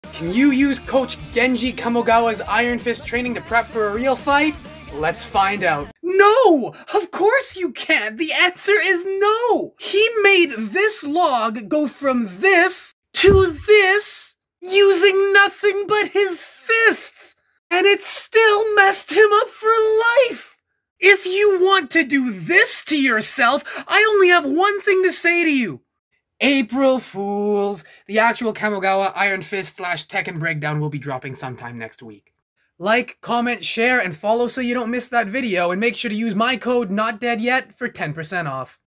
APRIL FOOLS: Can you use Coach Genji Kamogawa's Iron Fist Training to prep for a real fight?
Can you use Coach Genji Kamogawa's Iron Fist Training to prep for a real fight? Let's find out! 🔥🥊

April Fools